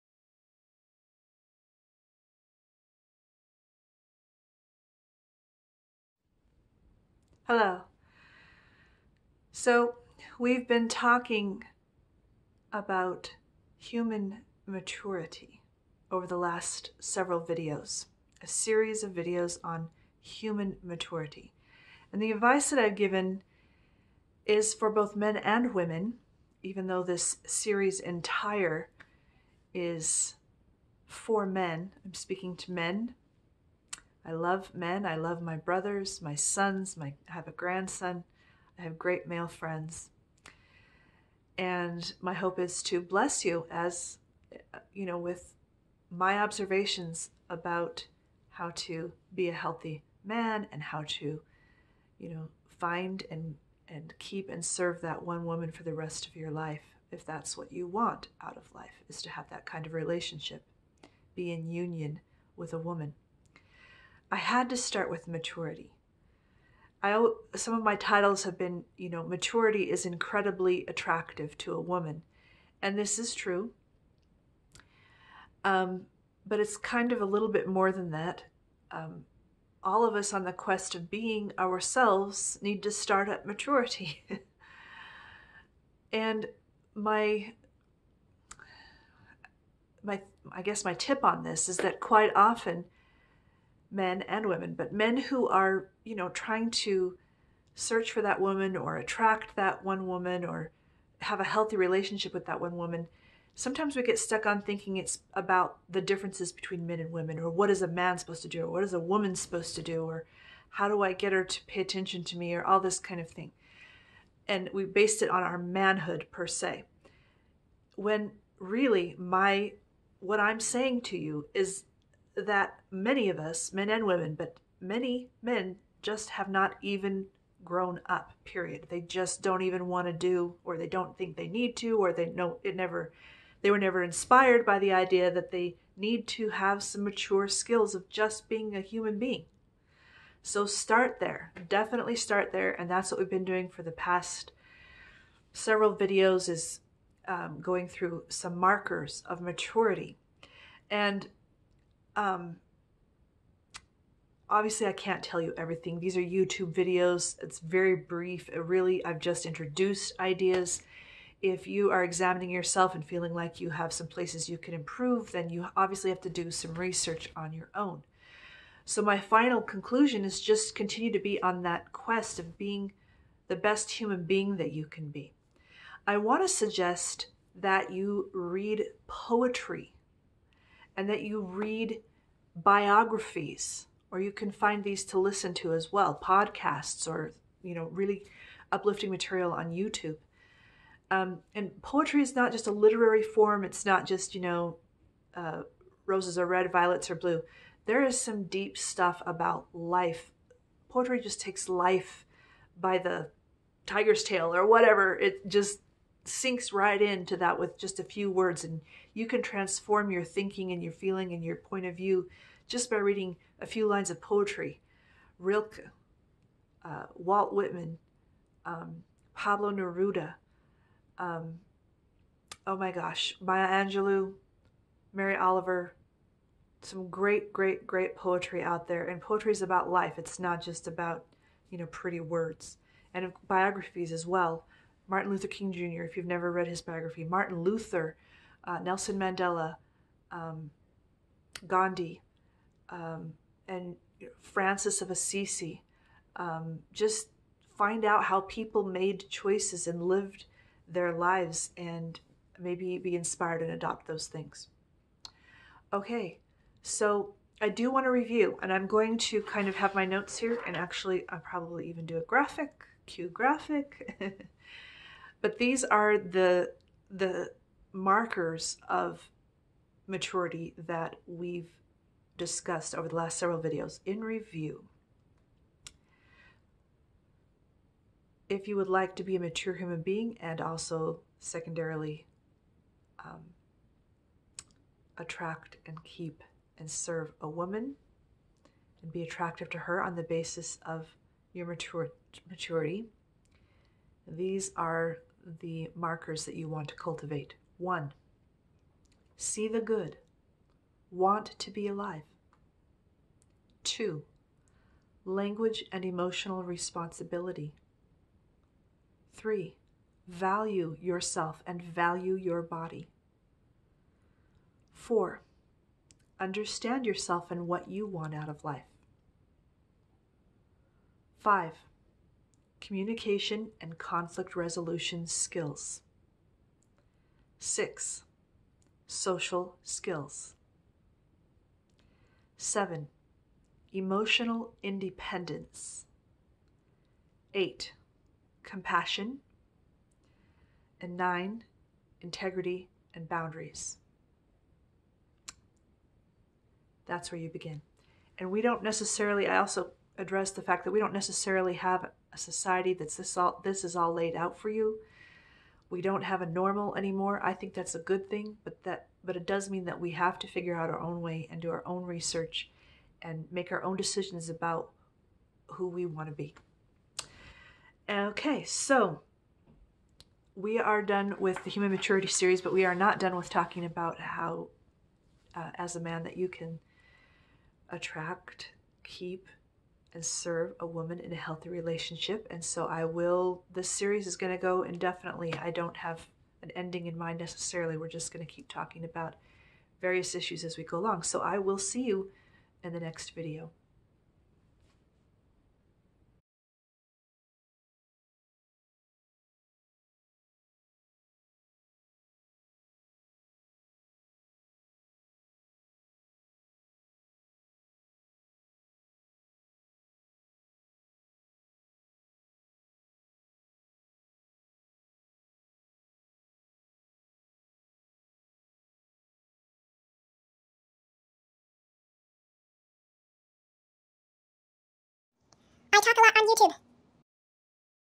Human Maturity Conclusion and Review (For Men 12)
Be on the quest for maturity and human-ness.  Conclusion and review of the human maturity series portion.

Read poetry: Rilke, Pablo Neruda, Maya Angelou, Mary Oliver, etc

Read biographies:  Martin Luther King, Jr; Nelson Mandela, etc

Image by StockSnap on Pixabay (modified by me).

[For Men: How To Attract, Keep, and Serve a Woman]